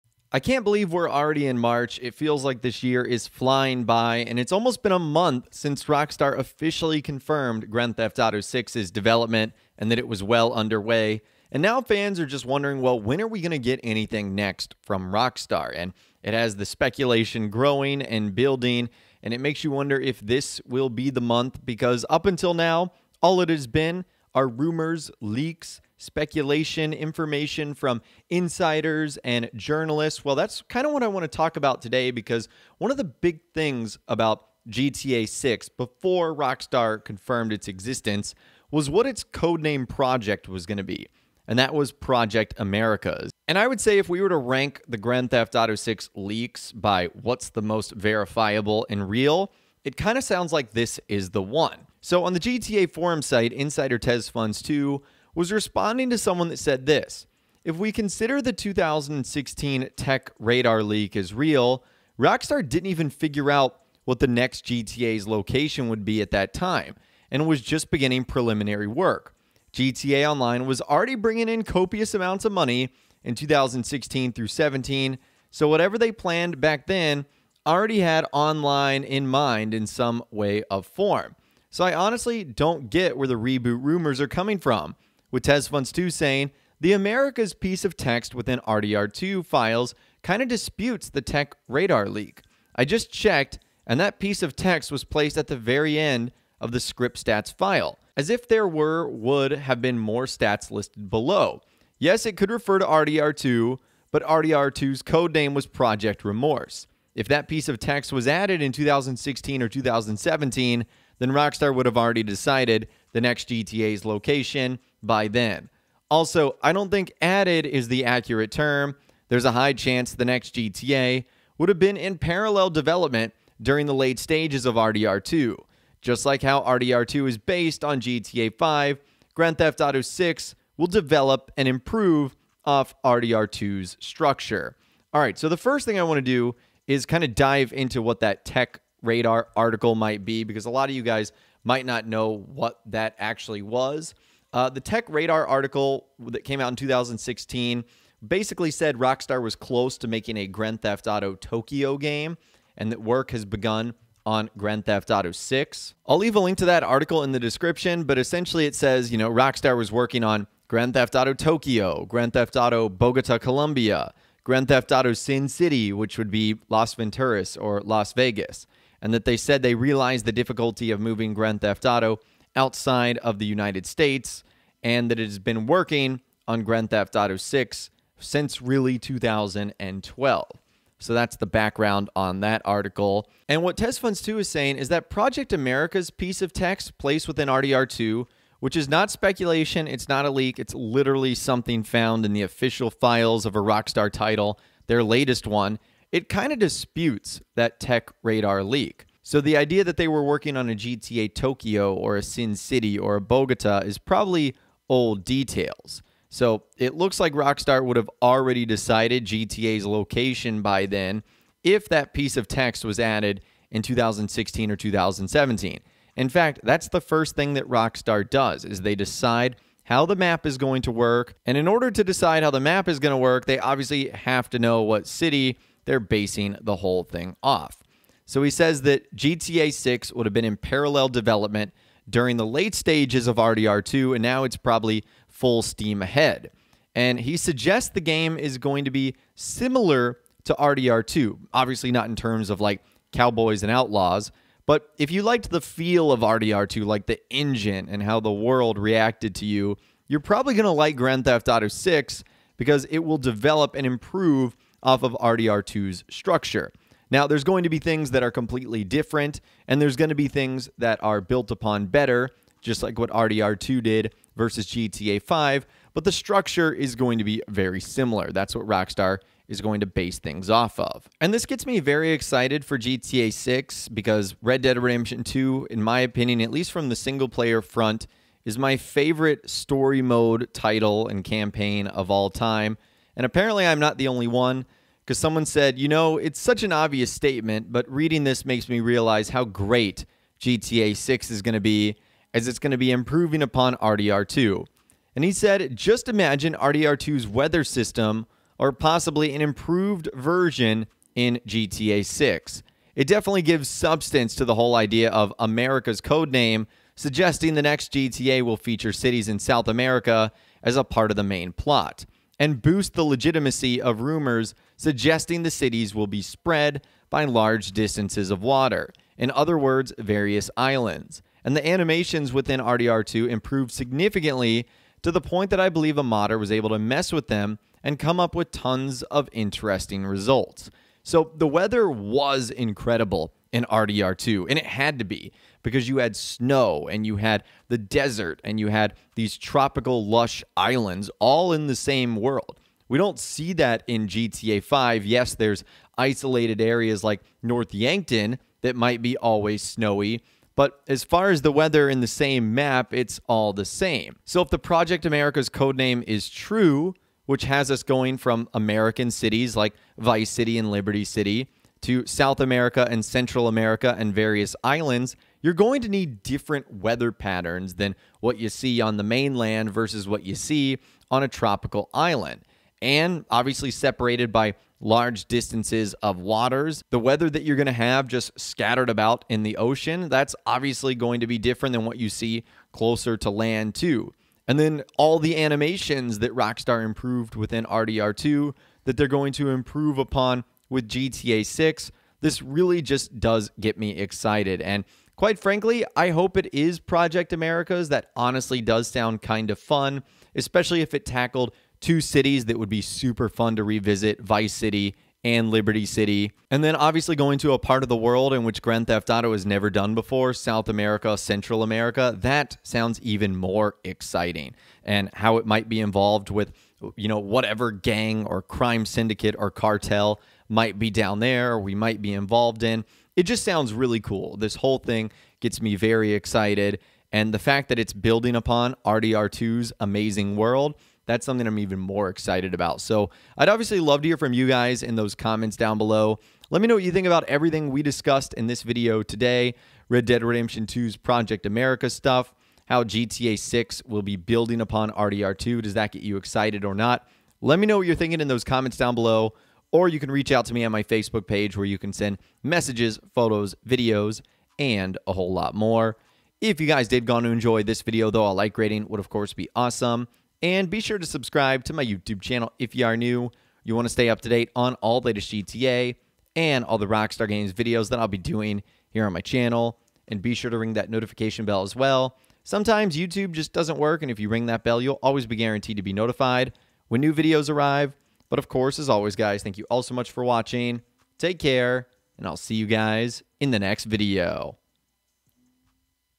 Grand Theft Auto 6: Americas - NEW Islands, Multiple Cities, Rockstar's Plan & MORE! (GTA 6)
In today’s Grand Theft Auto VI video – Grand Theft Auto 6: Americas - NEW Islands, Multiple Cities, Rockstar's Plan & MORE! (GTA 6)